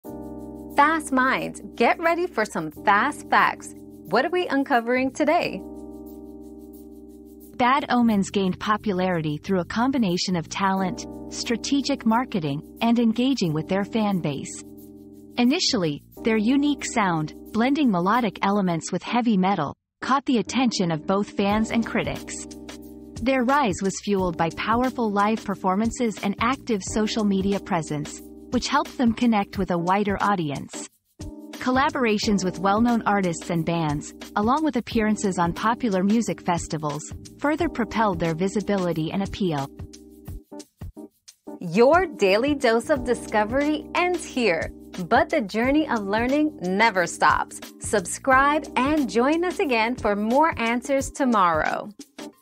How did Bad Omens get popular?
The Rise of Bad Omens: How They Became a Music Sensation 👉 The Rise of Bad Omens 👉 Discover how Bad Omens became a sensation in the music industry, combining talent, marketing strategies, and fan engagement to captivate audiences worldwide. From their unique sound to powerful live performances and collaborations with renowned artists, find out what made Bad Omens so popular.

Laura S. Harris (2024, February 11.) How did Bad Omens get popular?
    🌐 AskAbout.video